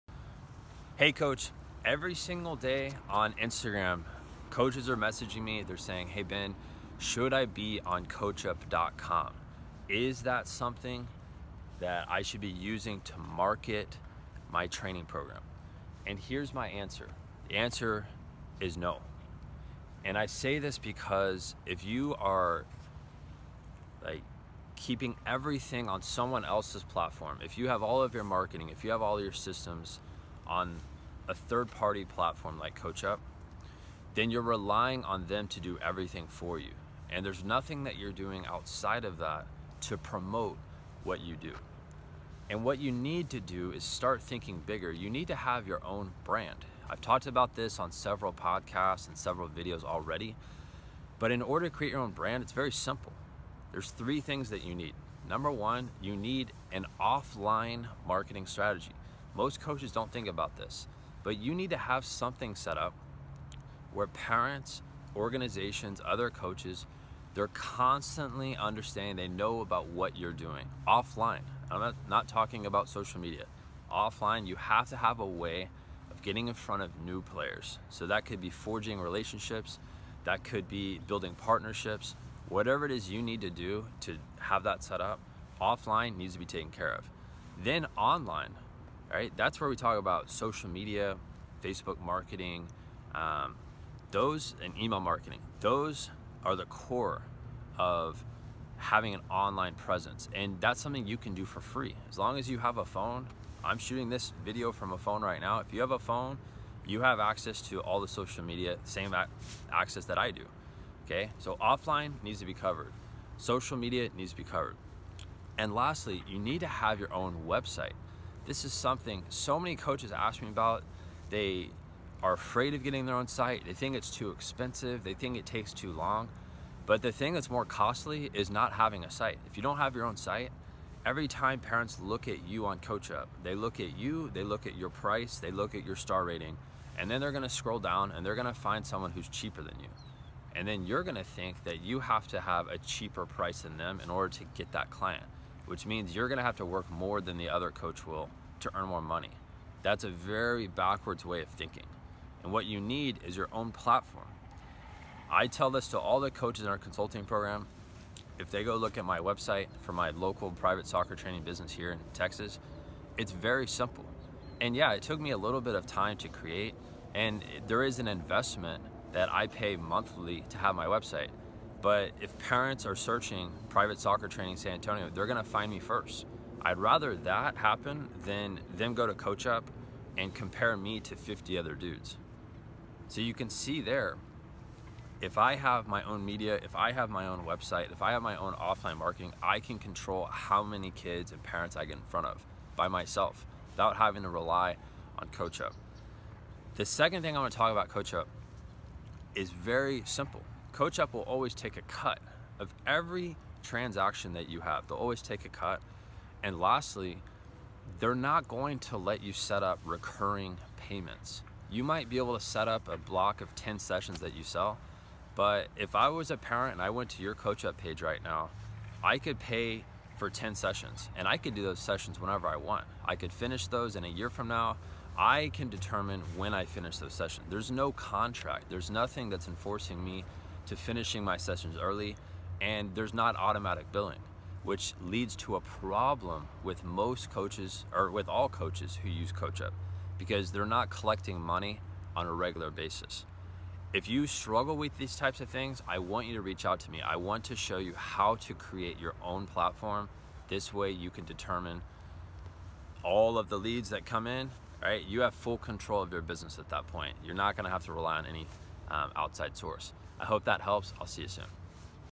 CoachUp.Com (Beware of this) | Sports Business Vlog #15
Should I use Coachup.com? In today's video, Coach Ben breaks down why you should not use coachup for your business.

Chat soon,
Coach Ben